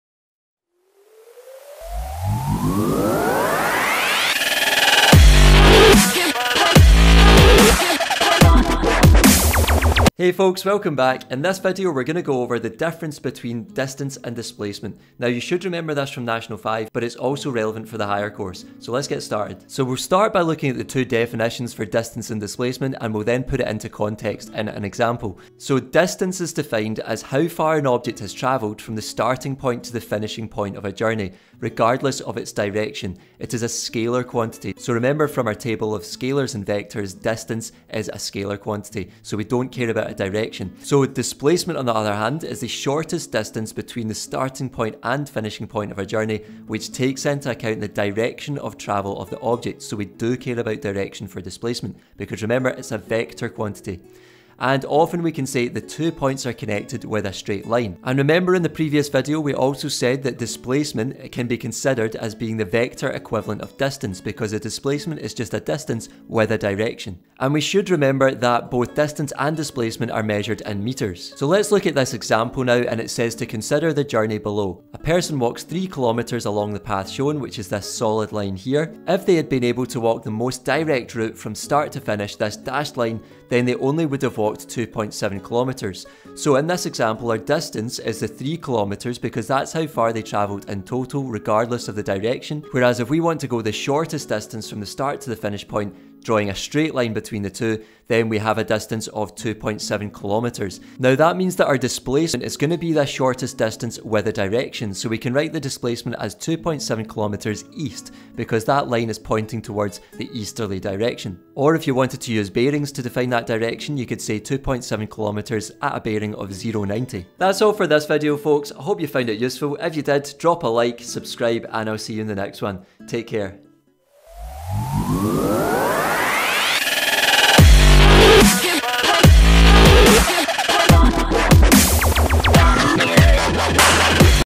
Higher Physics | Our Dynamic Universe | Distance & Displacement | THEORY
A brief reminder of the difference between distance and displacement.